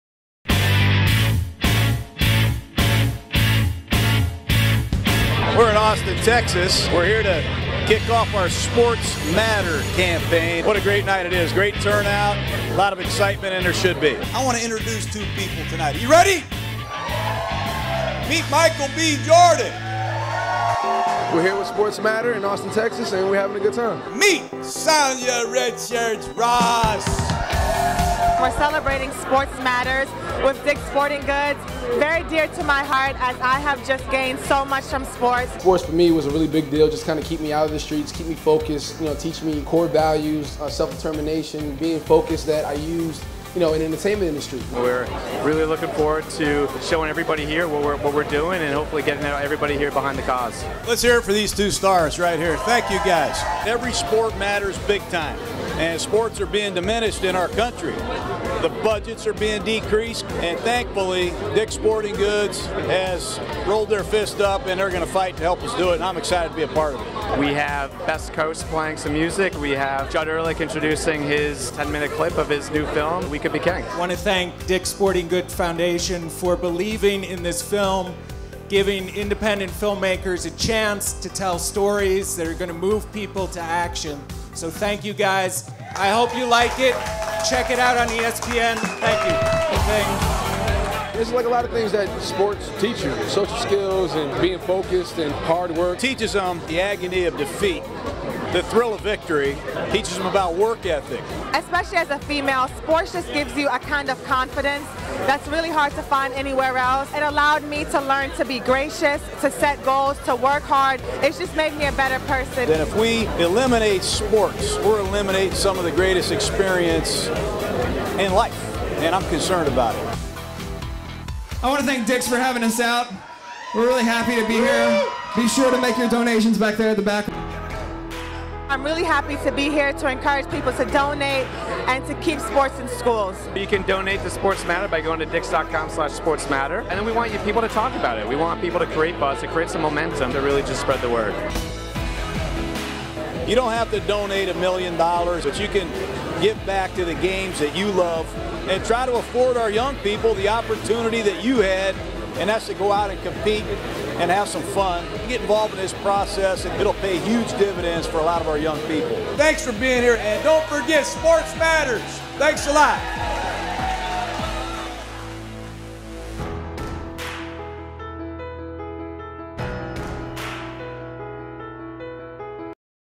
DICK'S Sporting Goods: Sports Matter- Launch Party at SXSW
The Sports Matter program launched with a party at SXSW including football analyst Jon Gruden, actor Michael B. Jordan, and Olympic gold medalist Sanya Richards-Ross sharing with attendees how sports had shaped who they are today.